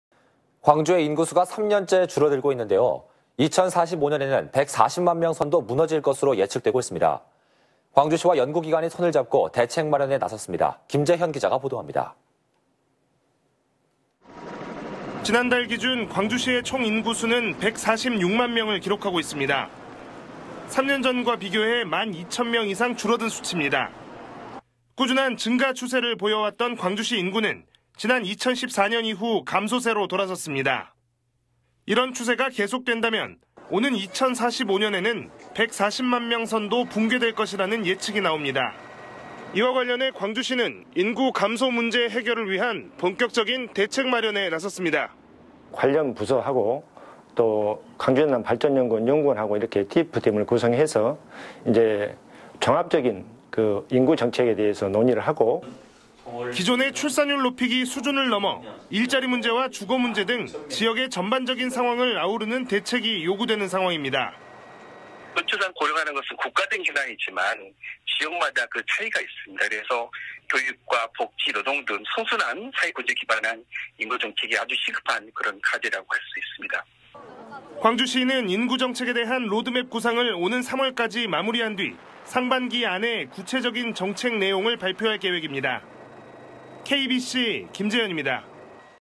인구 감소세 '뚜렷' 대책 마련 잰걸음
광주의 인구수가 3년째 줄어들고 있는데요. 2천 45년에는 140만 명 선도 무너질 것으로 예측되고 있습니다.

광주시와 연구기관이 손잡고 대책 마련에 나섰습니다.

김재현 기자가 보도합니다.